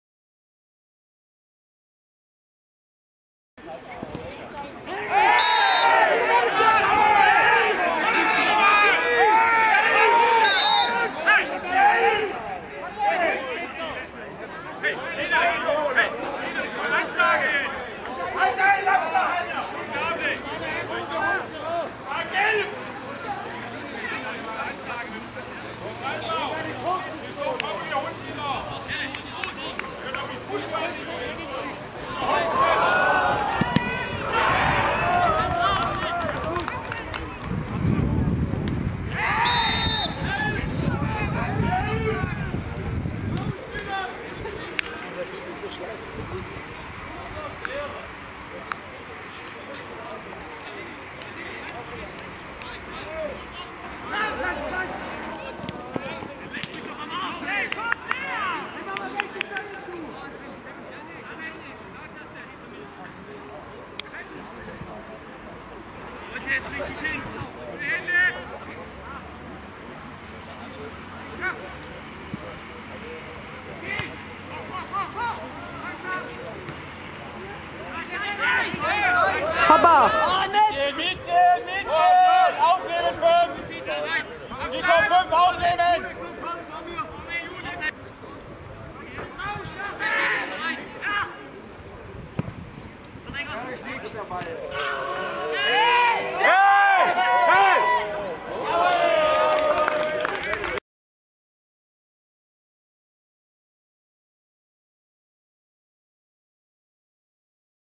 mössingen in nagold 3
highligthts vom spiel der mössinger jungs
in nagold, viele chancen und kein tor    :-(